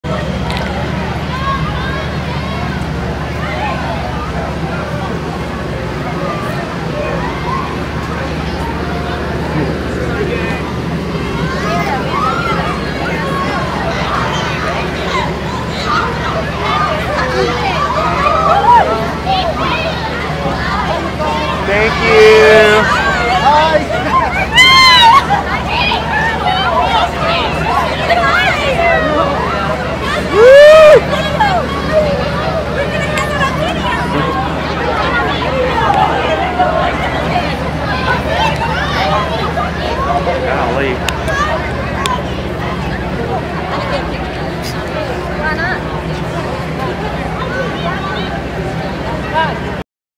keith urban and Nicholle sighting after CMT 2010
keith urban and Nicholle sighting after CMT 2010 Awards Nashville